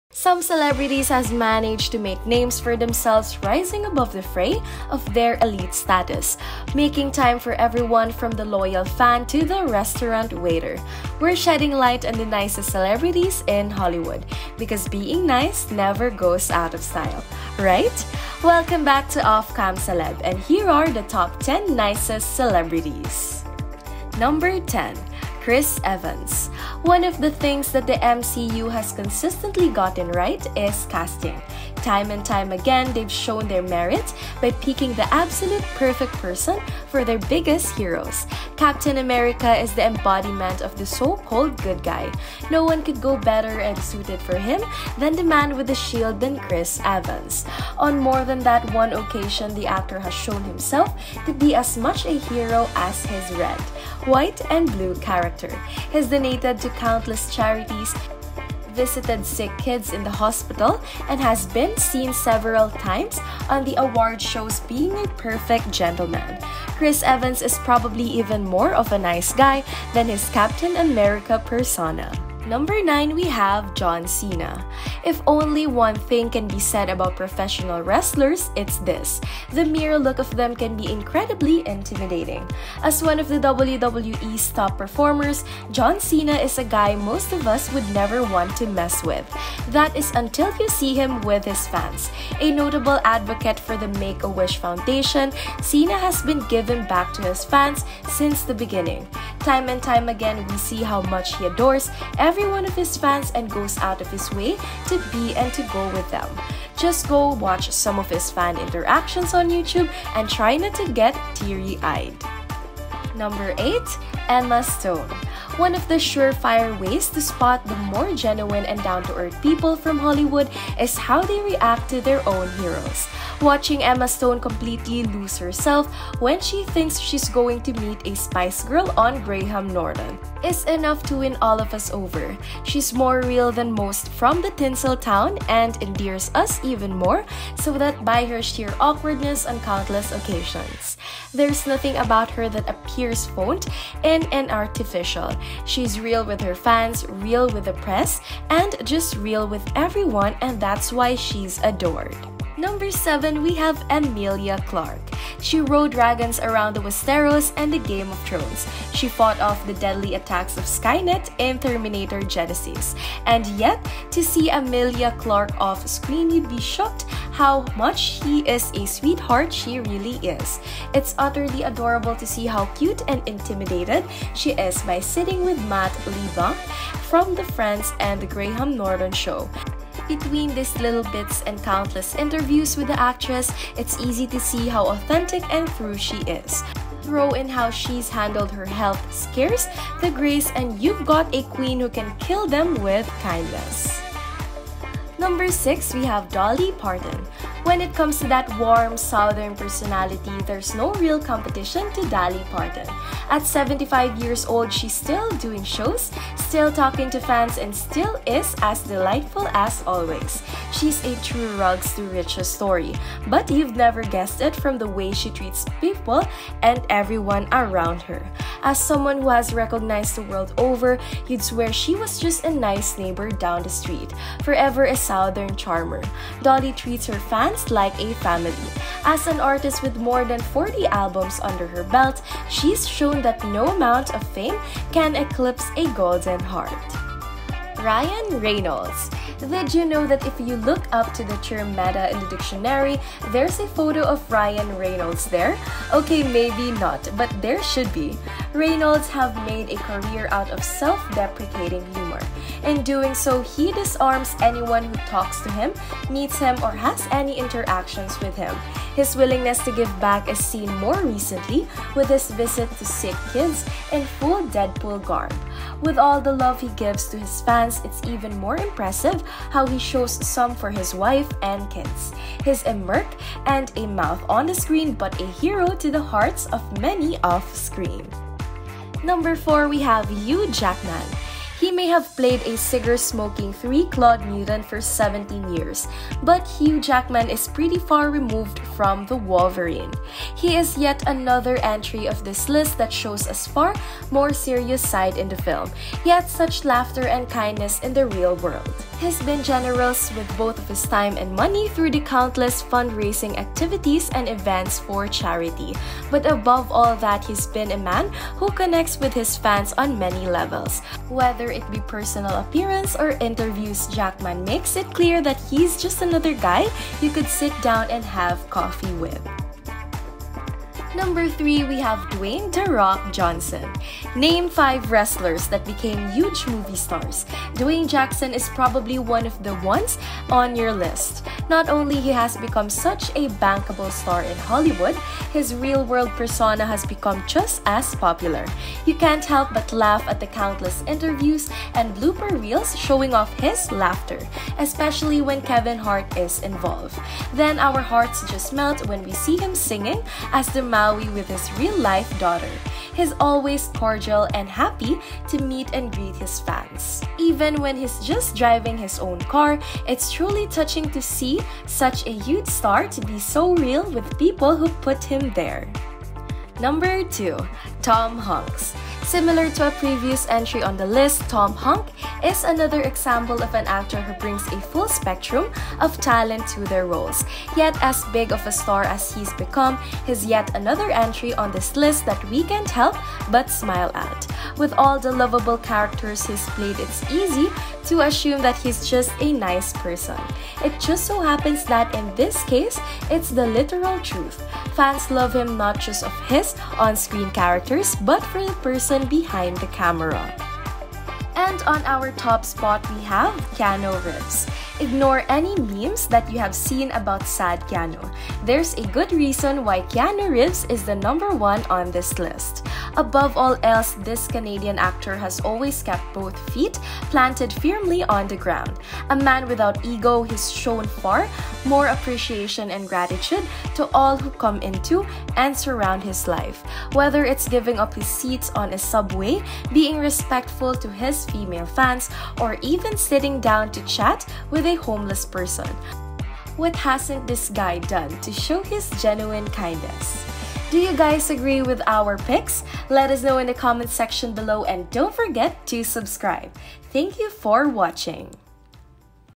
Top 10 Nicest Celebrities (Emma Stone, Keanu Reeves, John Cena & MORE!)
When it comes to fame, some celebrities make it a point to be as down-to-earth and unpretentious as possible. Here are the top 10 nicest celebrities of all time! (Keanu Reeves, Tom Hanks, Dwayne “The Rock” Johnson, Hugh Jackman, Ryan Reynolds, Dolly Parton, Emelia Clark, Emma Stone, John Cena, Chris Evans)